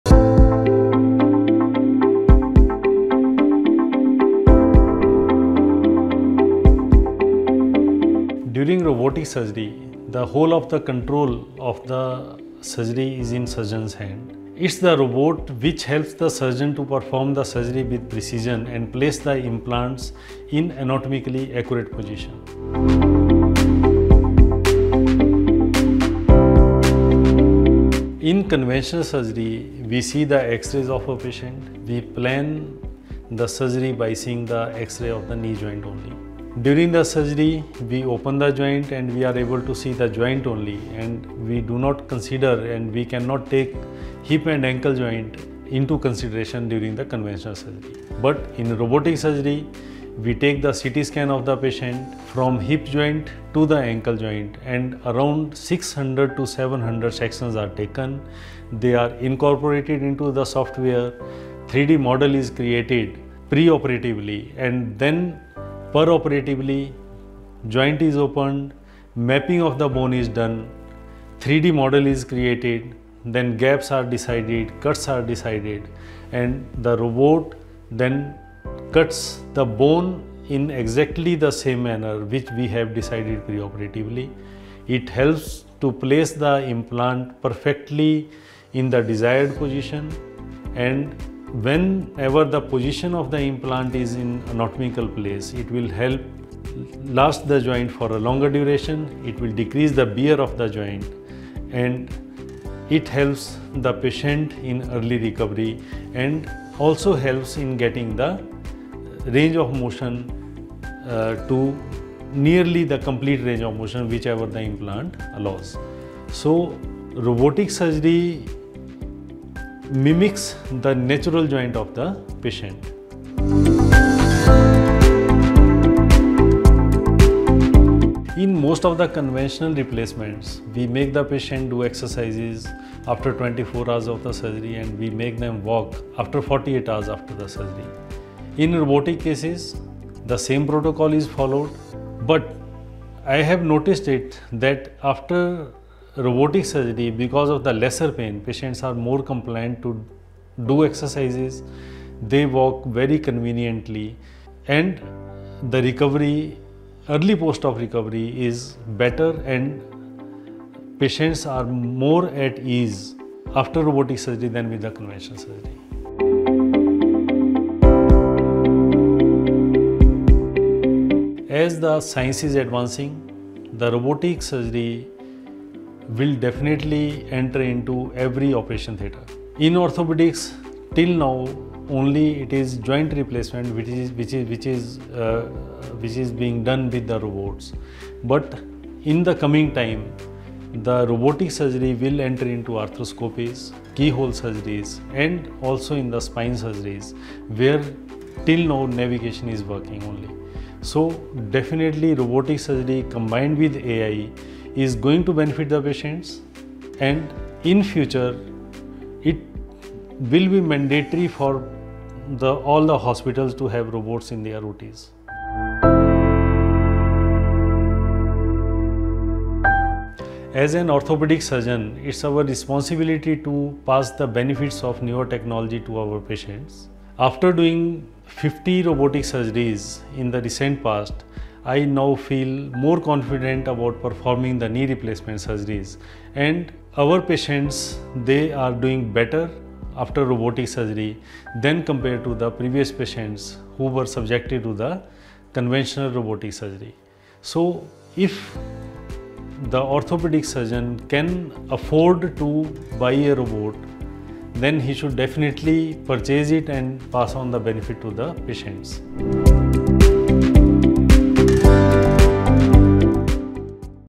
The Future of Joint Replacement with Robotics | Dr. Arun Chandel Shares Breakthrough Insights
🦾 What role is robotics playing in transforming joint replacement surgery?

In this expert discussion, Dr. Arun Chandel, an orthopedic specialist, explains how robotic-assisted joint replacement procedures are driving surgical accuracy and improving patient recovery outcomes.

Whether you’re a surgeon, med-tech professional, or a patient looking to understand the benefits of robotic joint replacement, this is a must-watch.

🔍 In this video:
✔ What is robotic joint replacement?
✔ How does it improve precision and outcomes?
✔ Clinical insights from Dr.Arun Chandel
✔ Meril’s contribution to robotic innovations in orthopedics